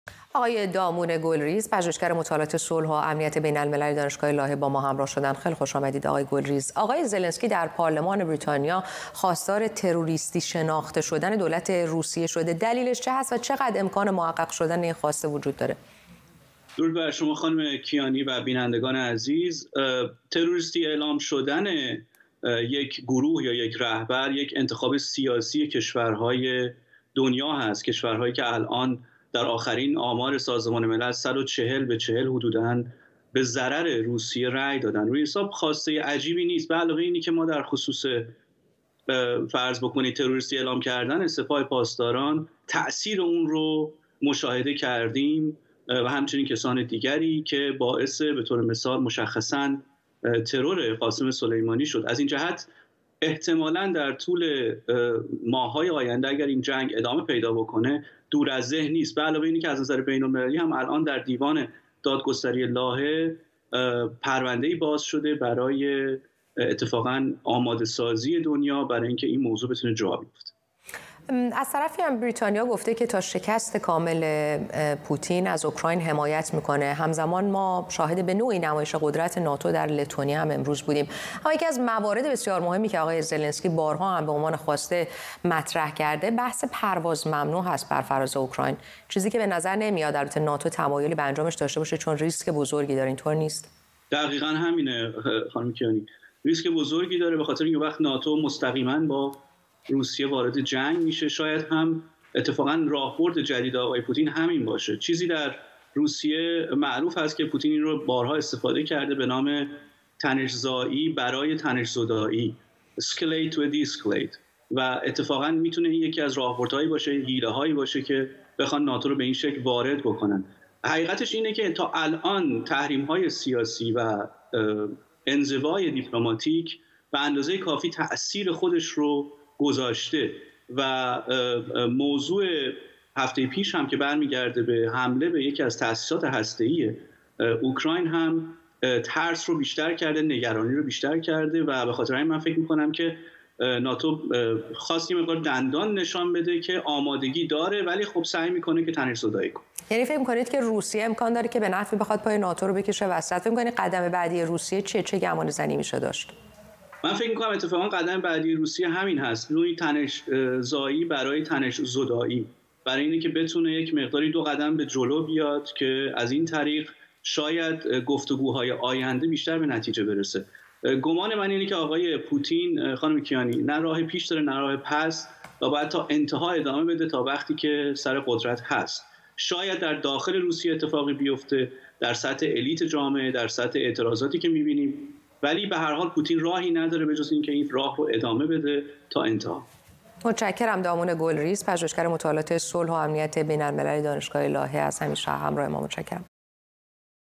ولودیمیر زلنسکی، رئیس‌جمهوری اوکراین خواستار تروریستی شناخته شدن دولت روسیه شد
گفت‌وگو با دامون گل‌ریز، پژوهشگر مطالعات صلح و امنیت بین‌الملل دانشگاه لاهه